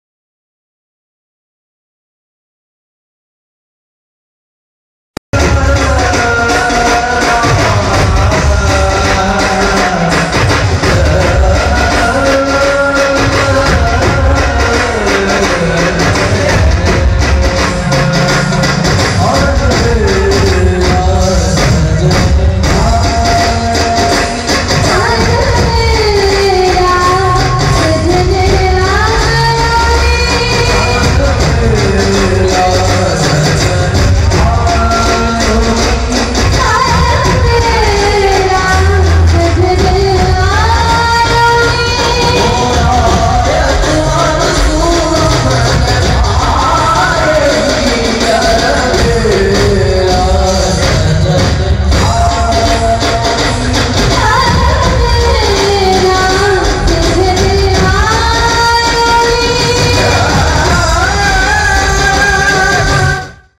Garbha gujarat ka(junagadh)
This vedio has taken at gunjan, junagadh, gujarat..
By shivam tiwari